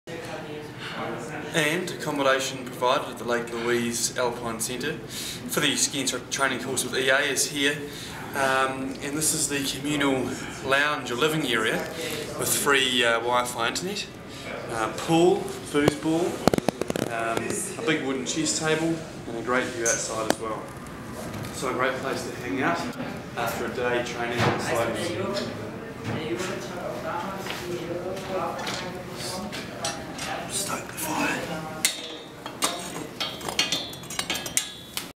Ski instructor course accomadation Lake Louise
Check out the sweet accommodation on a ski instructor course in Lake Louise with EA Ski and Snowboard